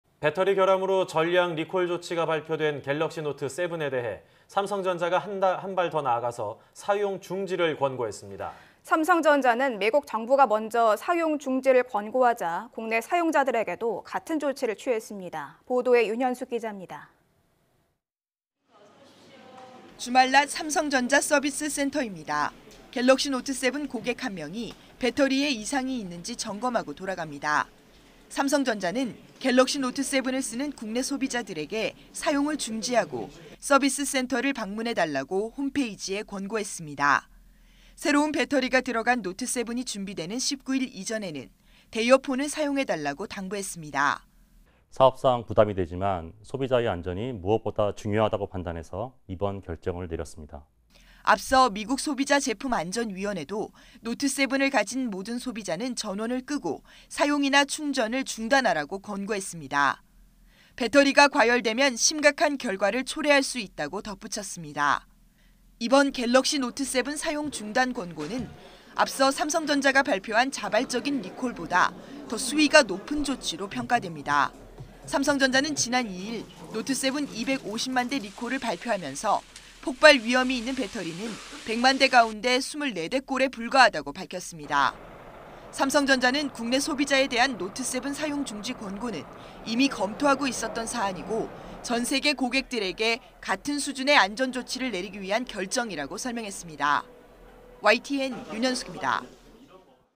삼성전자, 갤럭시 노트7 국내외 사용중단 권고 / YTN 사이언스
[앵커]
배터리 결함으로 전량 리콜 조치가 발표된 갤럭시 노트7에 대해 삼성전자가 이번에는 한 발 더 나가서 사용 중지를 권고했습니다.

미국 정부가 먼저 사용 중단을 권고하자 삼성전자가 국내 사용자들에게도 같은 내용의 조치를 취했습니다.

보도에 윤현숙 기자입니다.

[기자]
주말 낮 삼성전자 서비스센터입니다.

갤럭시 노트7 고객 한 명이 배터리에 이상이 있는지 점검하고 돌아갑니다.

노트7을 교환하러 온 이용자는 많지 않았습니다.

삼성전자는 갤럭시 노트7을 쓰는 국내 소비자들에게 사용을 중지하고 서비스센터를 방문해 달라고 홈페이지에 권고했습니다.

새로운 배터리가 들어간 노트7이 준비되는 19일 이전에는 대여 폰을 사용해달라고 당부했습니다.

[이준호 / 삼성전자 커뮤니케이션팀 부장 : 사업상 부담이 되지만 소비자의 안전이 무엇보다 중요하다고 판단해서 이번 결정을 내렸습니다.]

앞서 미국 소비자제품안전위원회도 노트7을 가진 모든 소비자는 전원을 끄고, 사용이나 충전을 중단하라고 권고했습니다.

배터리가 과열되면 심각한 결과를 초래할 수 있다고 덧붙였습니다.

이번 갤럭시 노트7 사용 중단 권고는 앞서 삼성전자가 발표한 자발적인 리콜보다 더 수위가 높은 조치로 평가됩니다.

삼성전자는 지난 2일 노트7 250만 대 리콜을 발표하면서 폭발 위험이 있는 배터리는 100만 대 가운데 24대꼴에 불과하다고 밝혔습니다.

삼성전자는 국내 소비자에 대한 노트7 사용 중지 권고는 이미 검토하고 있었던 사안이고 전 세계 고객들에게 같은 수준의 안전 조치를 내리기 위한 결정이라고 설명했습니다.